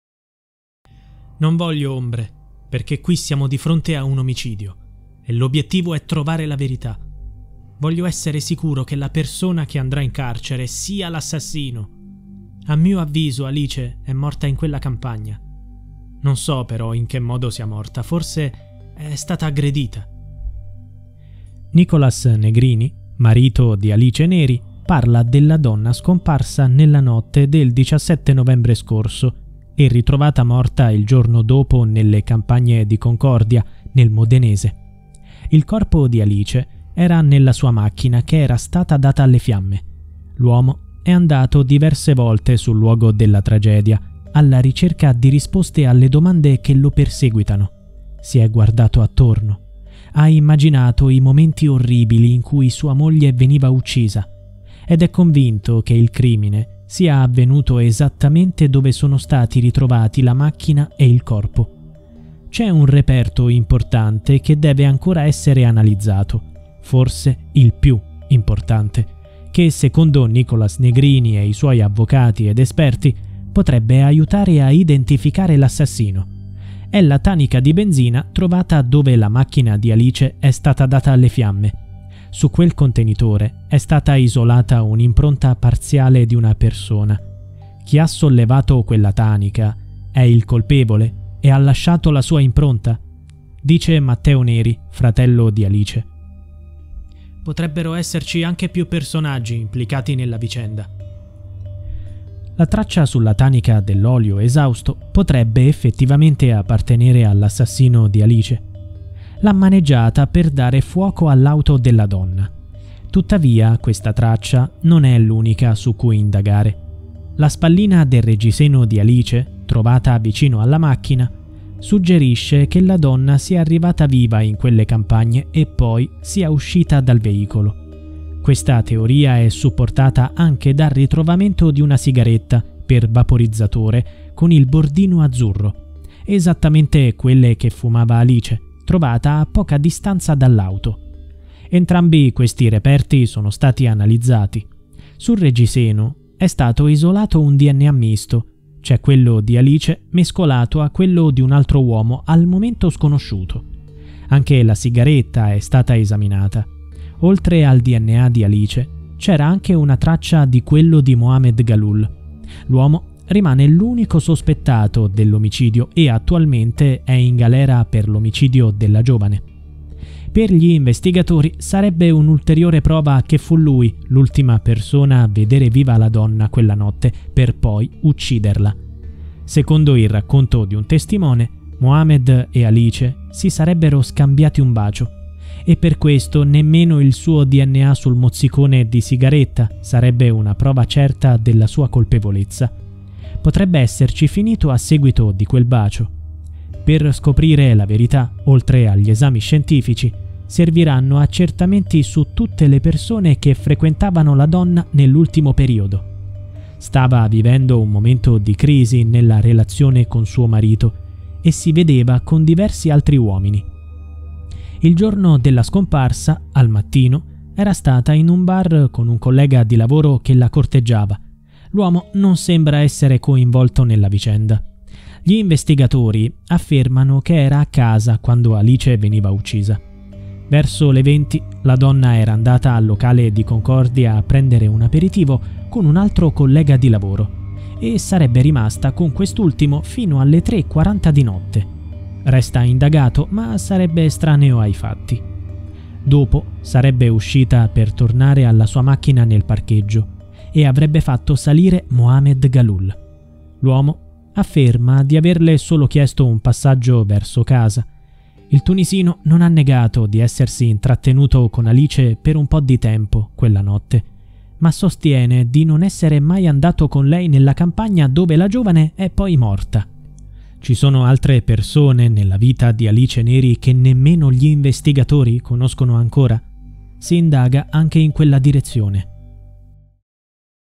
Trovato anche il DNA di Gaaloul - Alice Neri
Si indaga ancora sul delitto della donna bruciata nel Modenese